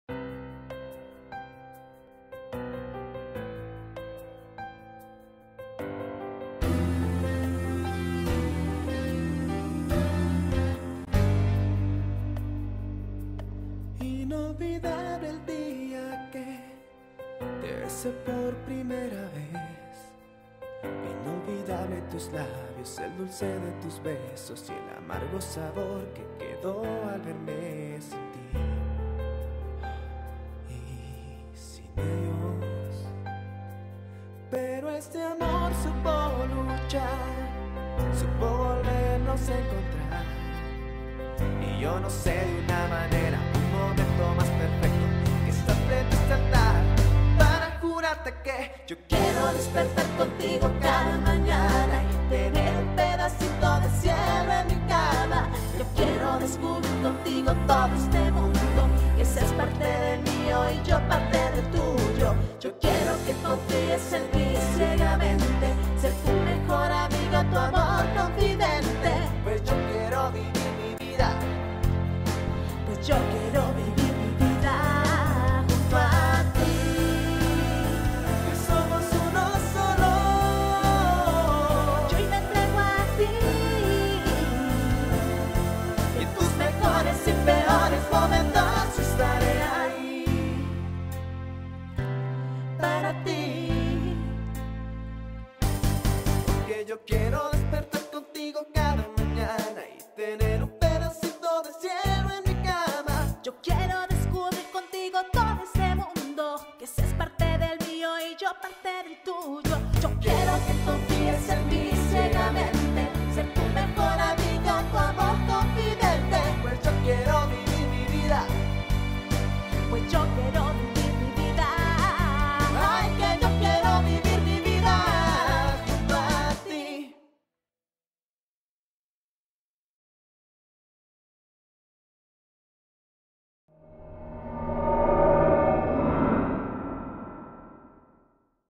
Highlights Boda Oaxaca: Mariana y Edgar
NOTA: TAMBIÉN PUEDES DISFRUTARLO EN HD** con la opción 720p o 1080p

Boda grabada en Tuxtepec, Oaxaca y editada por SB Producciones